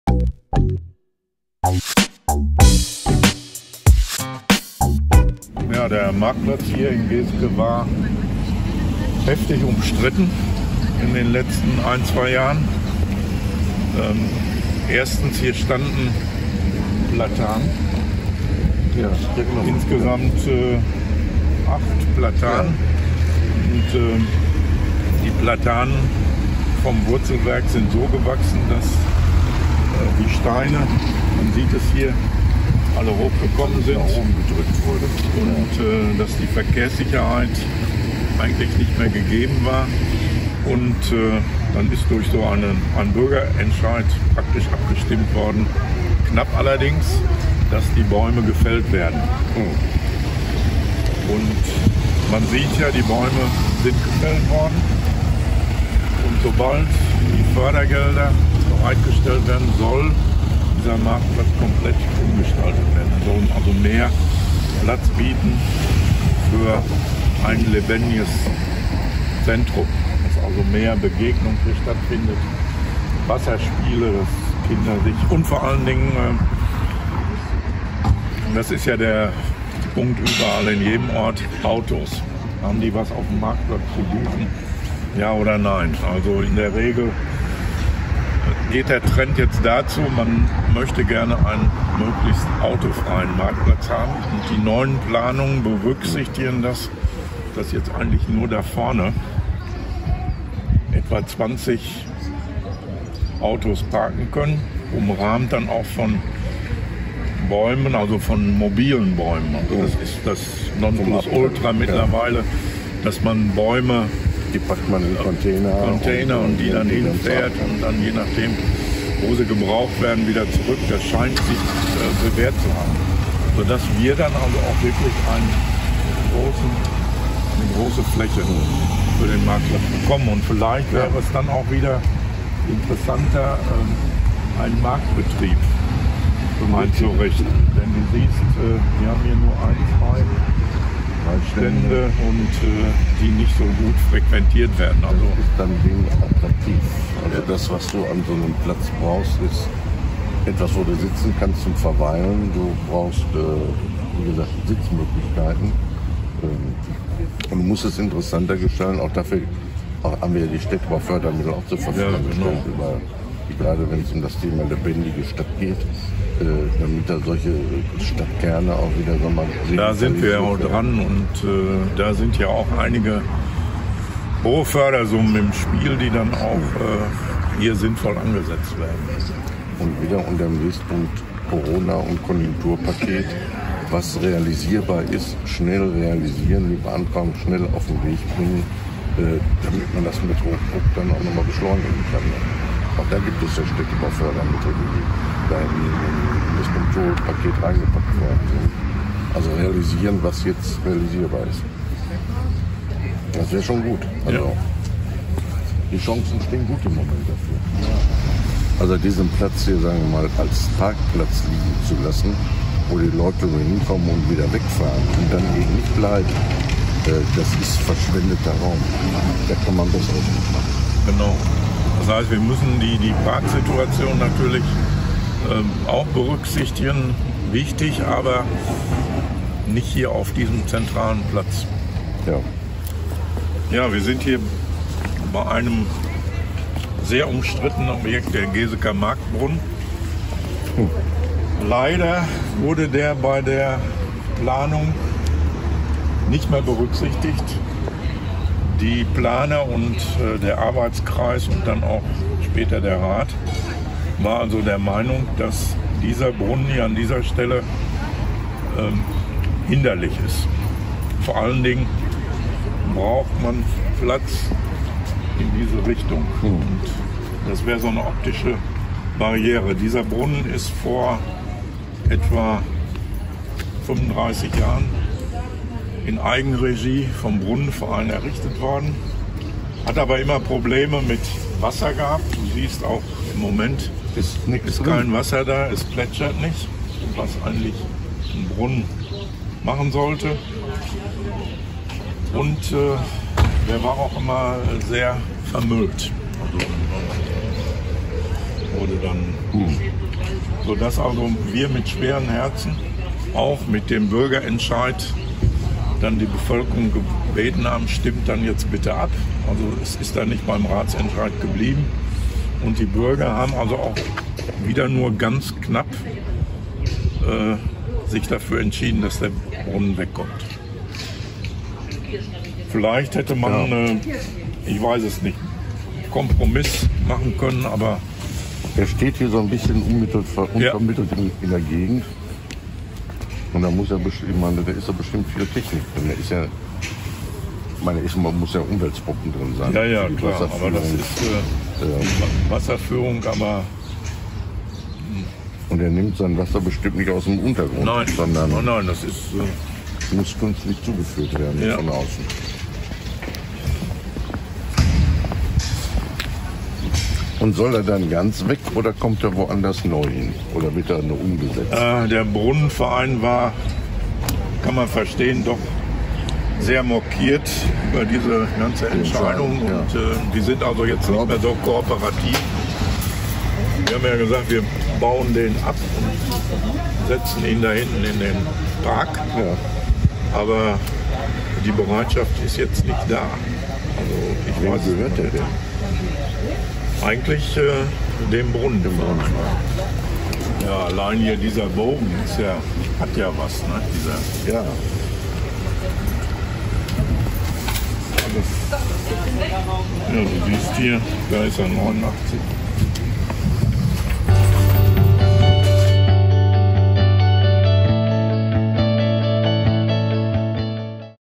Der Marktplatz in Geseke
Günter Fiedler und ich haben uns den Marktplatz in Geseke mal genauer angeschaut - warum er keine Bäume mehr hat und ob der Marktplatz als Parkplatz eine gute Idee ist, gibt es im Video zu sehen.